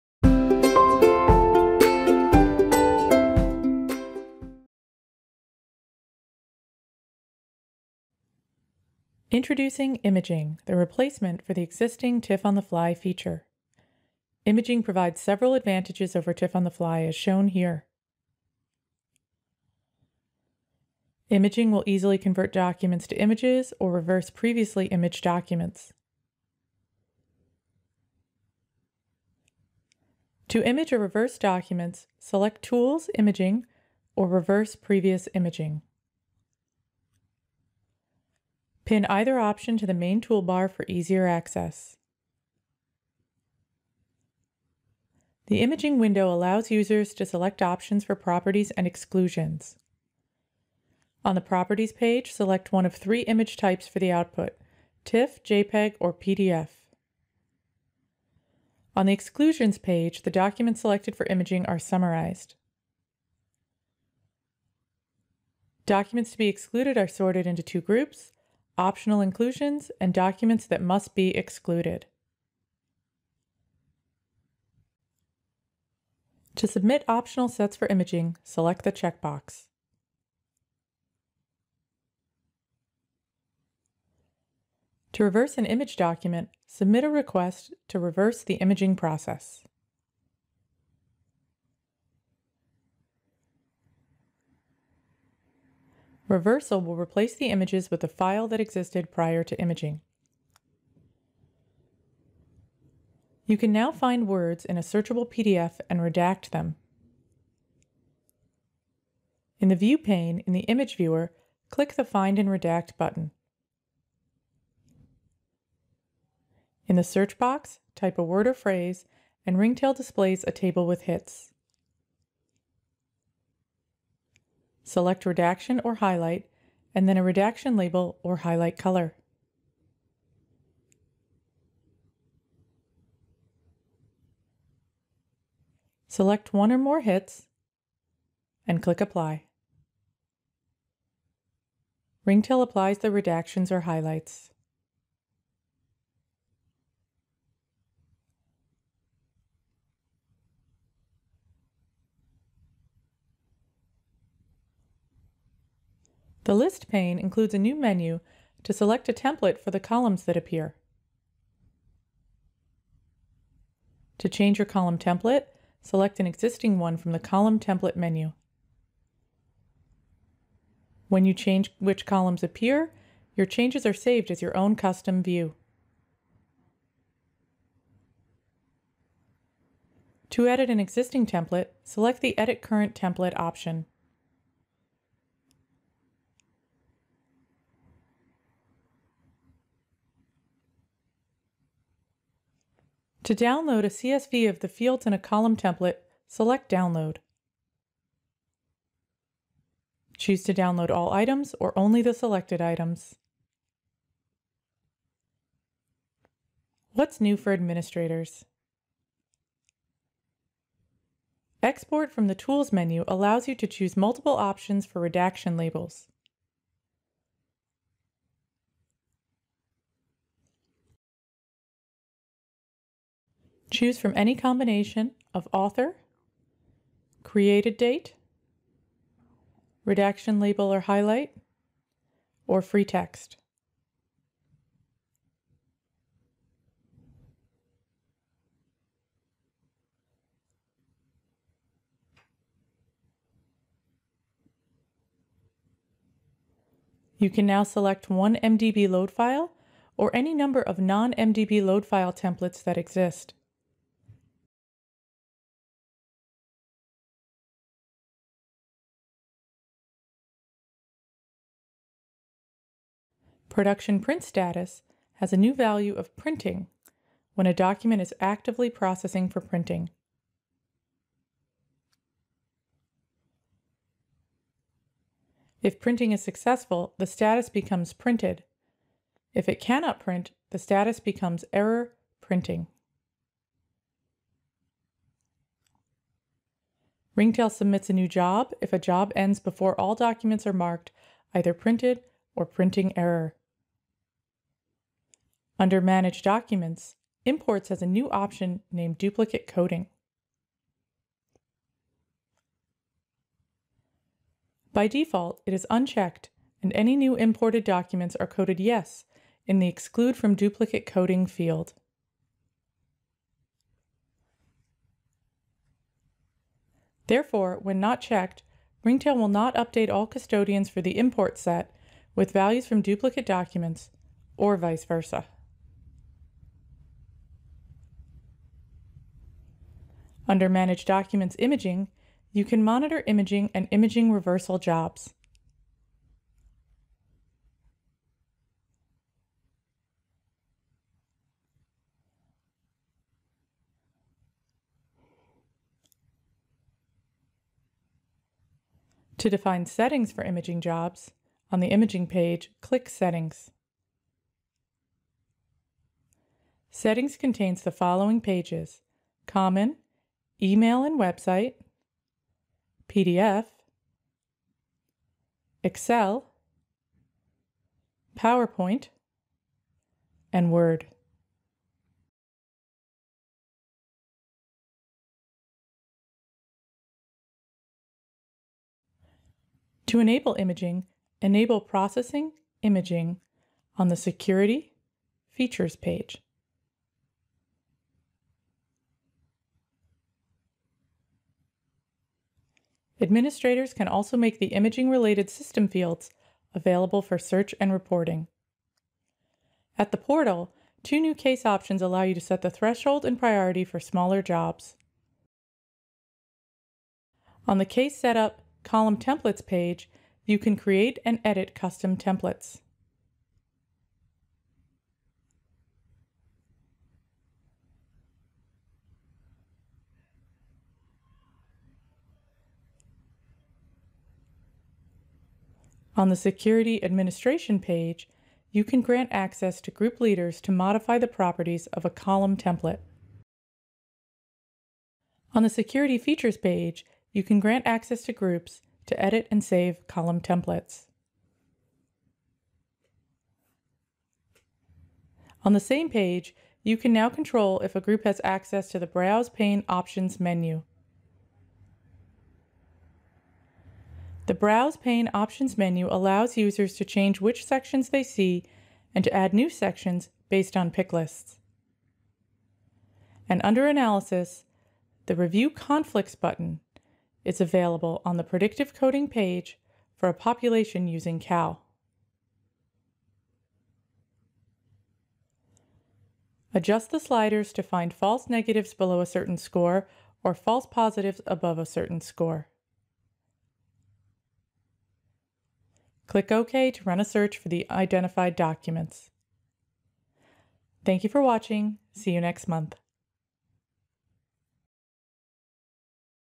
What's New In Ringtail? January 2018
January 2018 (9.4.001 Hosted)

What’s new for all users
Introducing the Imaging feature for users
Find and redact on Searchable PDFs
Column templates in the List pane

What’s new for administrators
Introducing the Imaging feature
Allow groups to edit Browse pane settings
Export from Search Results Enhancements
Predict Enhancements: Review Conflicts for CAL
Production Enhancements
Imports Enhancements: Duplicate Coding option